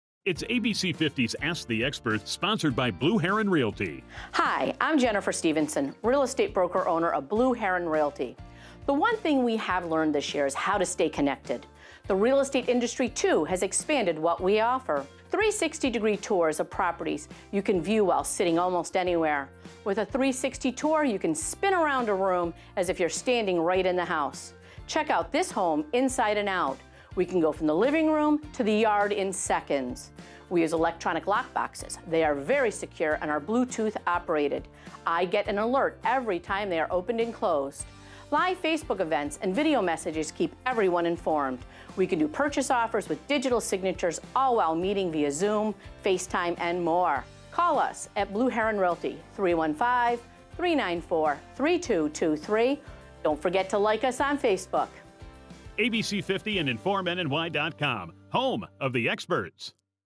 Ask the Expert: Blue Heron Realty discusses the virtual buying and selling process used during the p
Ask the Expert: Blue Heron Realty discusses the virtual buying and selling process used during the pandemic